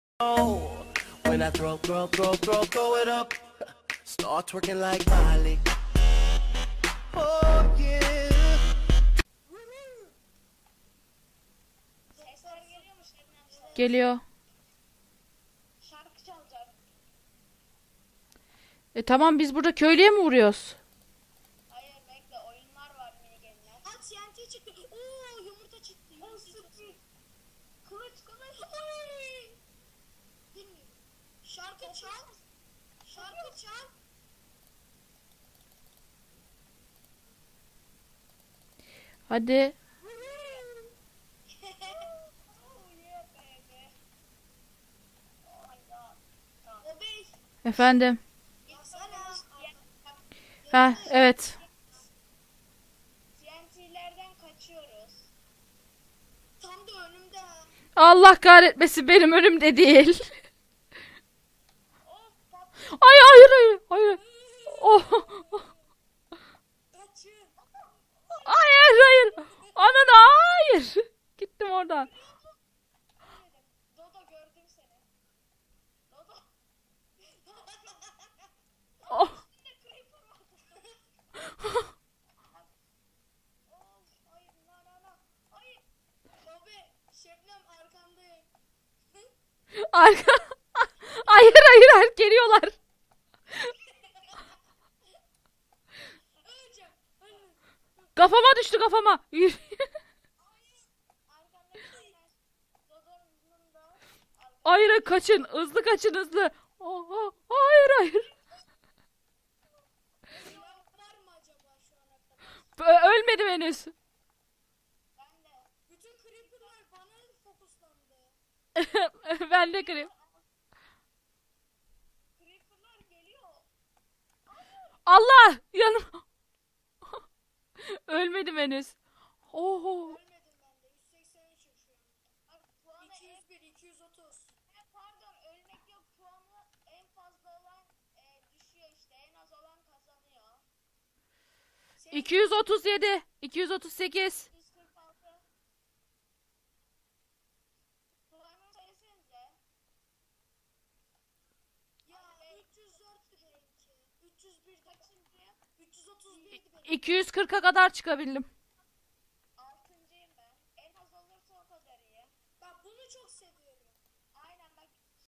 Mini Games Lab Giriş TNT den Gaçıyoz Gafama Düştü Gafama :D
arminin kanalı

tuqrulun kanalı

Tugrulla ve Armi ile Mini Games Oynadık :))